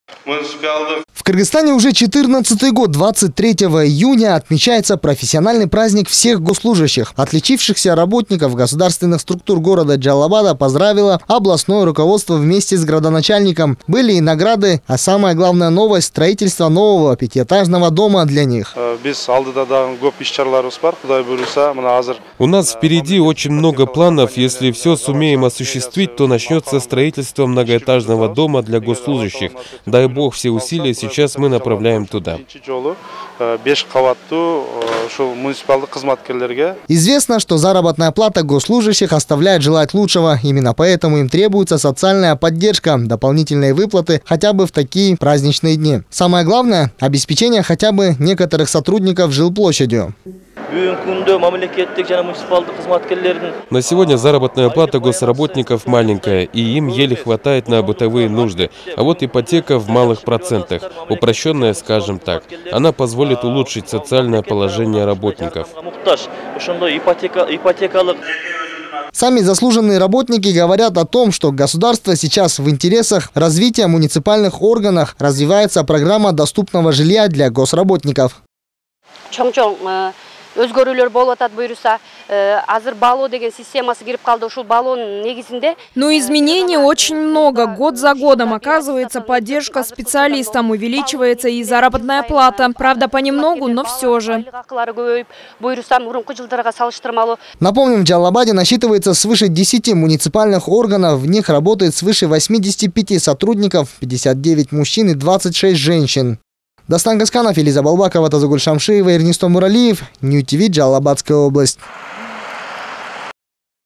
Госслужба- внимание и поддержка работникам
Завтра профессиональный праздник всех государственных служащих. В связи с чем в Джалал-Абаде для поддержания житейских условий госслужащих начнется строительство домов за счет упрощенной ипотеки. Все планы были озвучены на мероприятии, посвященном дню государственных и муниципальных работников.